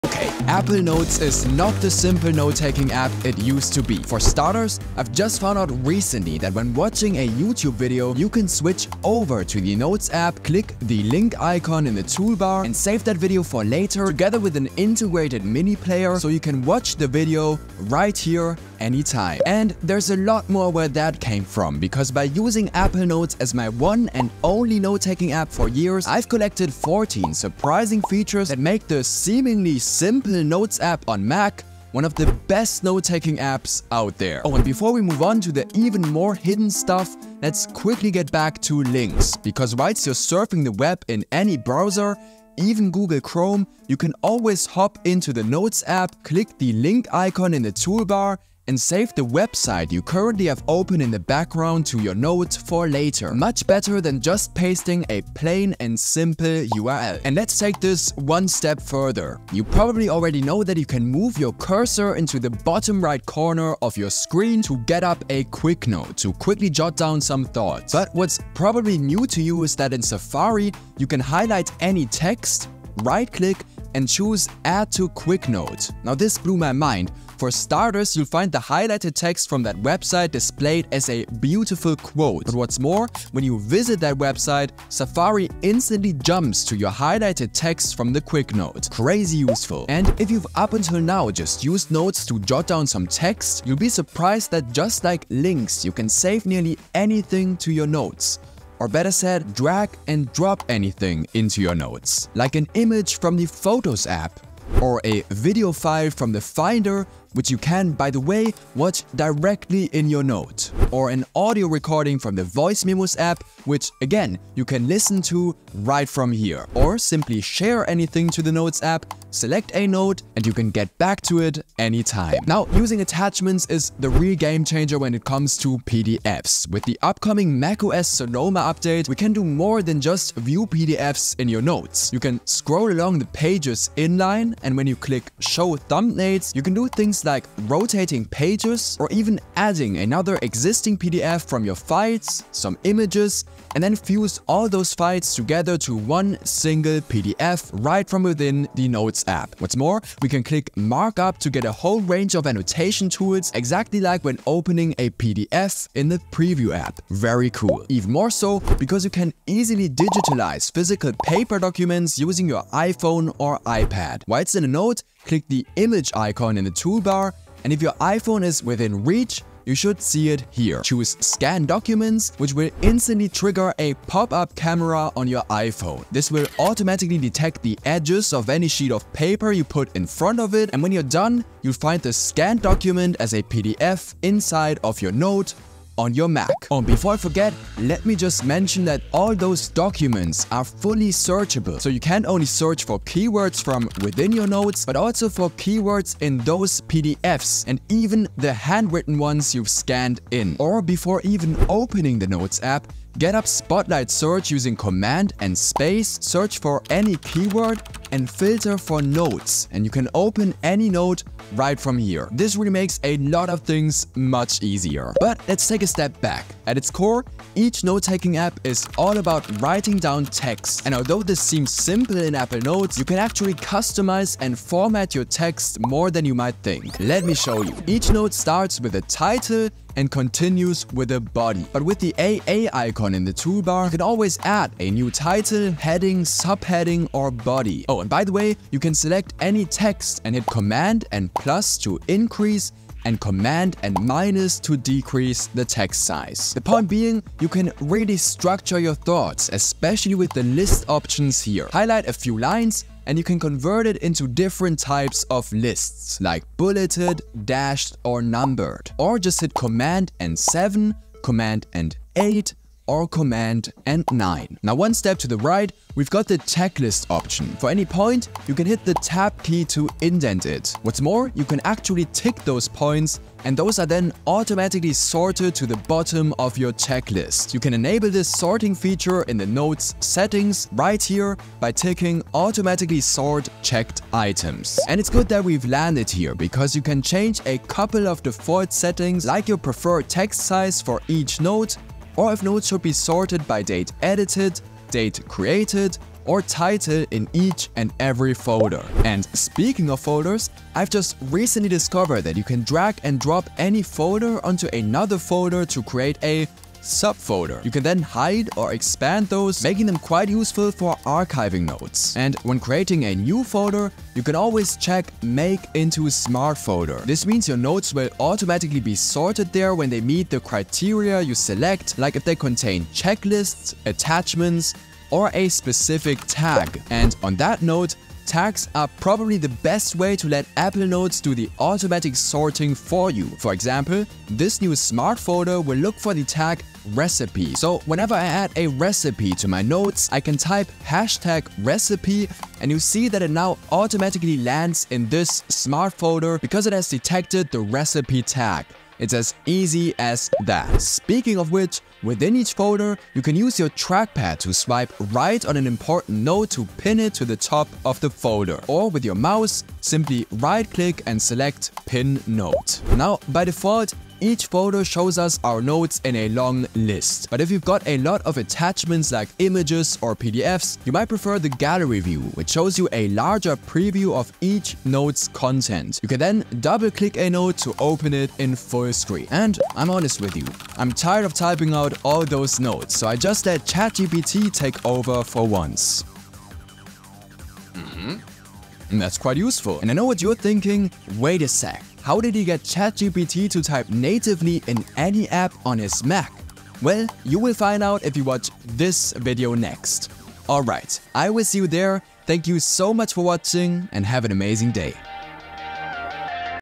How to (Actually) Use Apple Notes on Mac
Here’s how to get the most out of Apple Notes on your Mac! Discover 14 unbelievably useful Apple Notes tips & and tricks - including exciting new Apple Notes features coming with macOS Sonoma this fall.

📖 CHAPTERS 📖

00:00 - Add YouTube Videos
00:34 - Preview Links
00:57 - Save Quotes from Websites
01:29 - Add Audios, Videos and More
02:02 - Hidden PDF Annotation Features
02:37 - Scan Documents on Mac
03:06 - Smartest Search Ever
03:34 - Hidden Text Formatting
04:06 - Lists & Checklists
04:45 - Change Default Text Size
04:59 - Create Sub- & Smartfolders
05:26 - Easiest Way to Sort Your Notes
05:50 - Pin Important Notes
06:02 - Hidden (Pre)View Option
06:19 - ChatGPT in Apple Notes?!